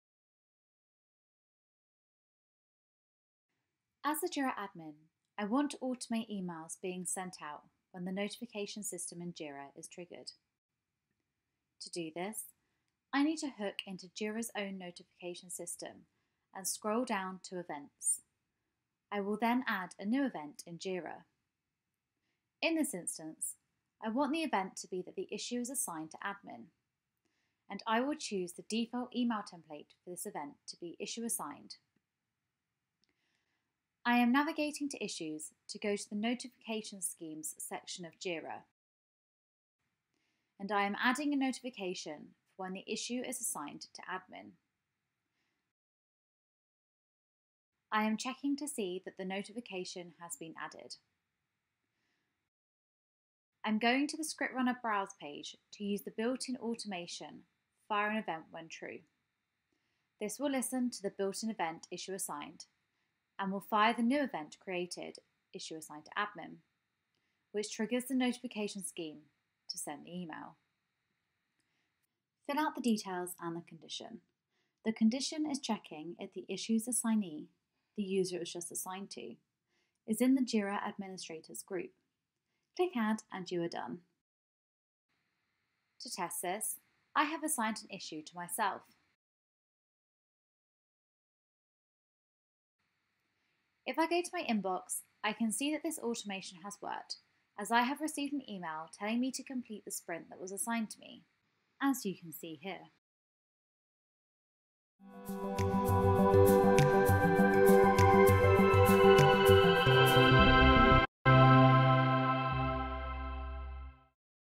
ScriptRunner for Jira | How to Automate Emails being Sent
ScriptRunner for Jira is great for automating your processes to save you time so you can focus on the things that matter. In this demo, we will be showing you how to automate emails being sent out when the notification system in Jira is triggered.